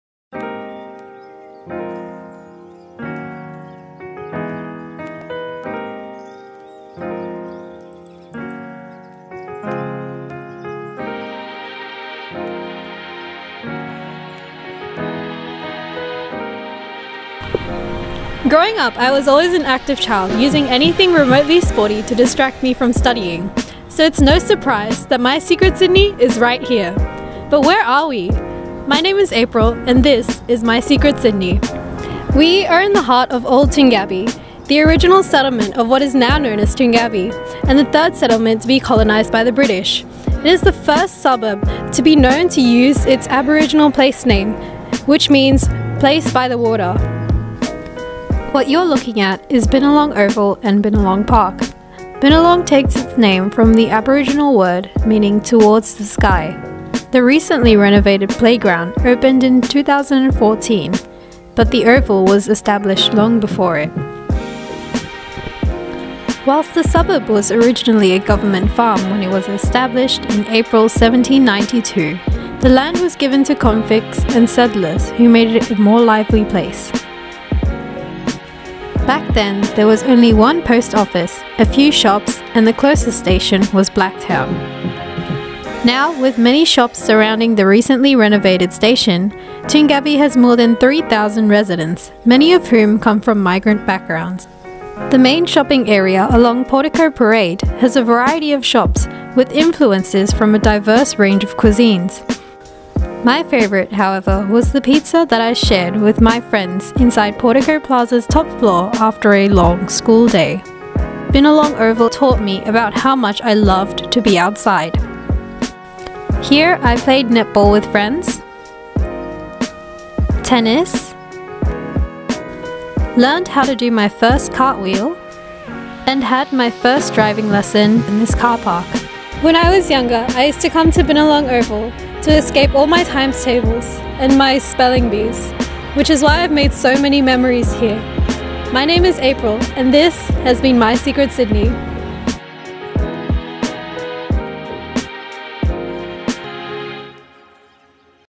Secret Sydney 61: Old Toongabbie
April Maung reports:
The quaint suburbs of Old Toongabbie and Toongabbie are the perfect mix of down to earth with sporty, filled with a vibrant history and enough cultural variety to keep everyone happy.